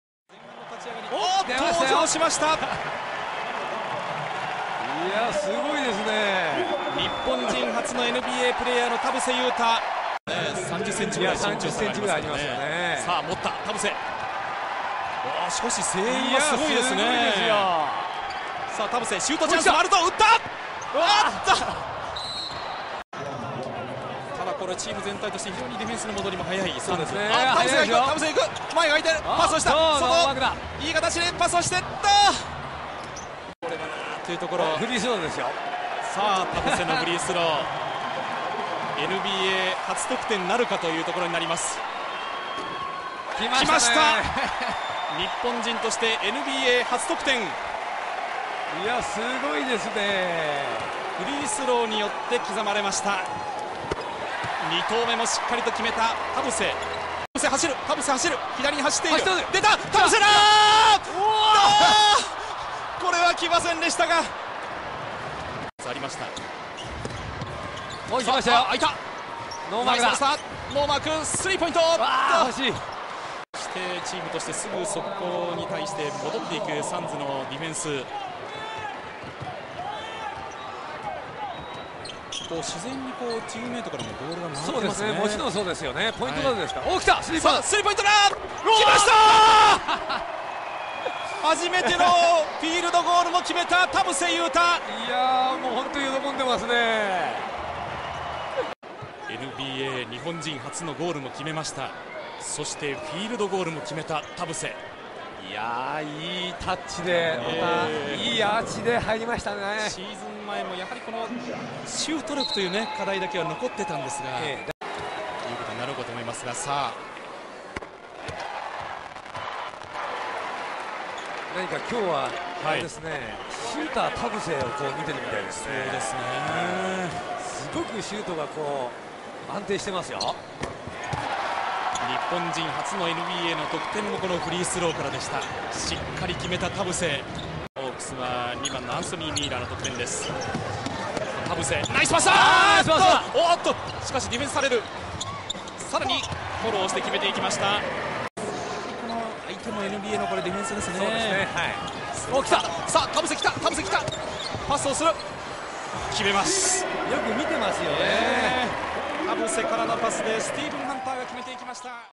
Yuta Tabuse NBA debut highlights -7pts 1ast 11.3.2004
説明田臥勇太NBAデビューのハイライト動画